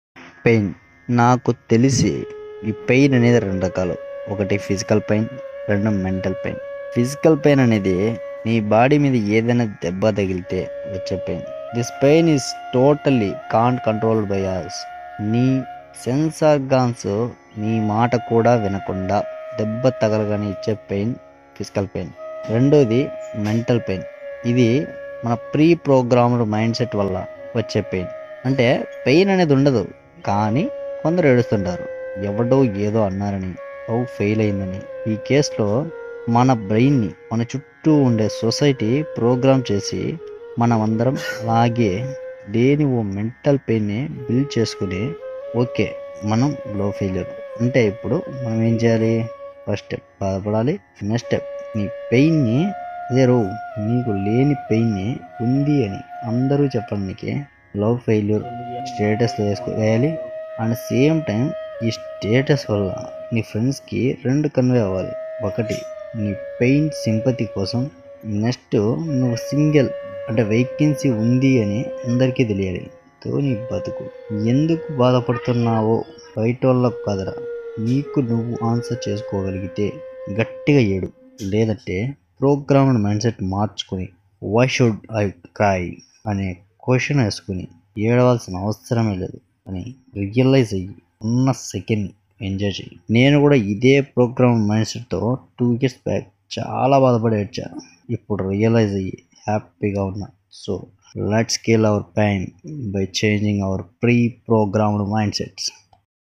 PAIN//AM LIKE YOU
Hello guys here we are discussing about how to think in different situations in different manner.
PREVIOUS VEDIOS LINK IN BELOW GUYS WATCH THIS VEDIOS ALSO.